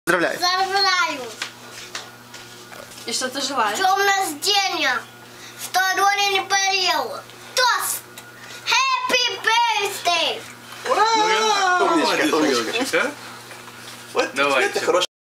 Aaron wishes Happy Birthday to Leonid
Classic - aaron wishes happy birthday on Leonid's 55th birthday at the catskil.  July 14th 20067